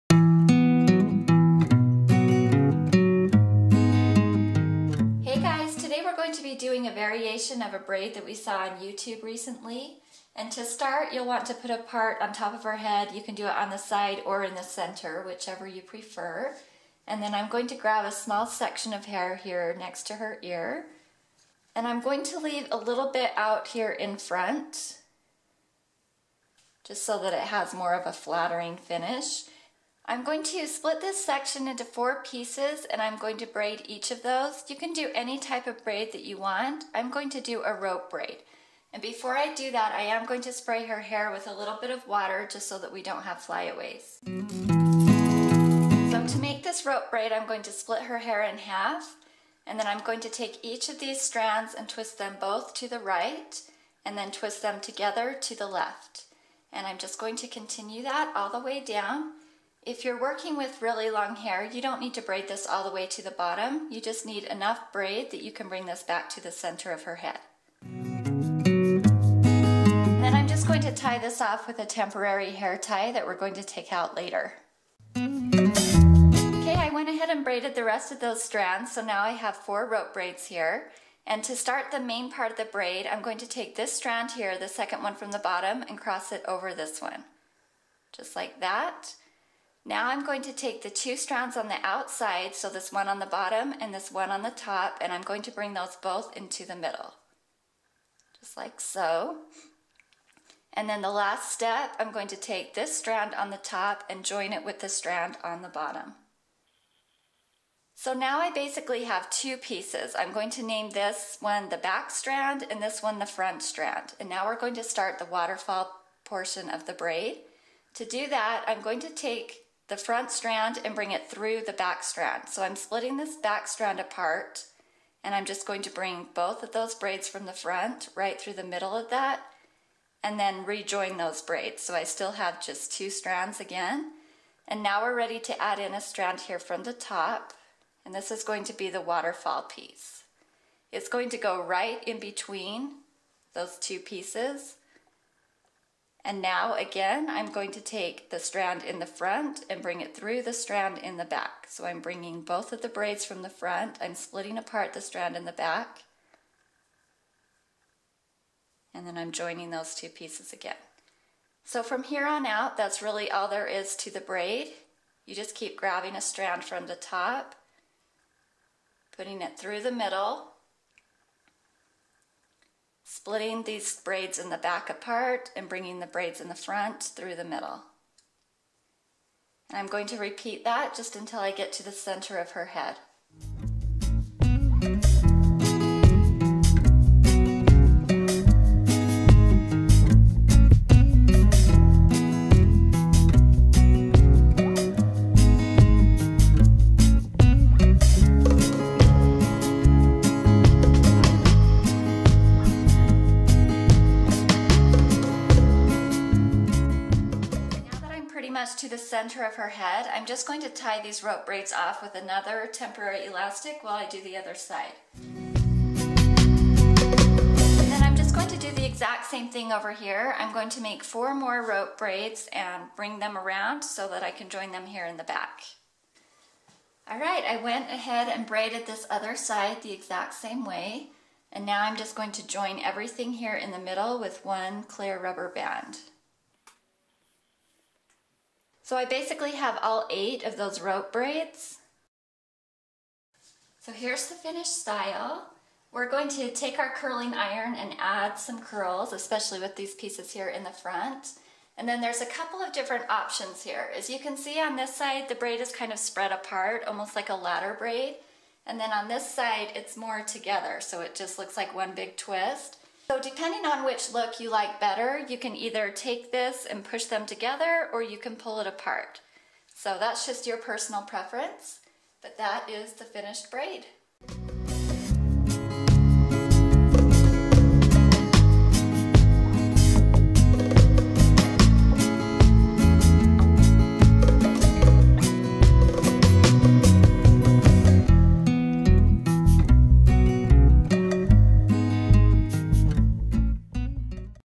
Scissor Twist Waterfall Braid Hairstyle
for hundreds of hairstyle ideas and tutorials.

This week our tutorial is inspired by a lovely braid we first saw on a YouTube channel "ViriYueMoon" called the "waterfall scissor braid." Our Princess has worn the original braid several times and loves it!

We wondered what this unique braid would look like if we added some "twists" or rope braids in place of the regular strands. We both ended up loving the outcome of this variation, so we wanted to share.

This tie back hairstyle has a similar look to a waterfall twist braid (or more accurately... a double waterfall twist braid.) But as you can see in our tutorial, the technique is quite different.

As shown towards the end of our video, you can spread the twists apart or keep them close together for 2 different looks.

FYI The curling wand we used to create these curls was our 19mm from NuMe.